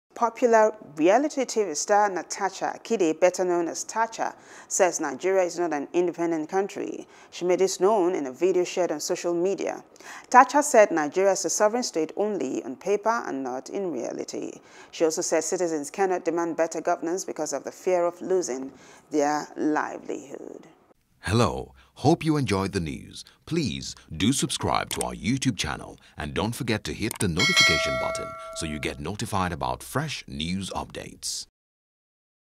Nigeria Is Not An Independent Country Tacha | ENTERTAINMENT
Popular reality TV star, Natacha Akide, better known as Tacha, says Nigeria is not an independent country. She made this known in a video shared on social media. Tacha said Nigeria is a sovereign state only on paper and not in reality.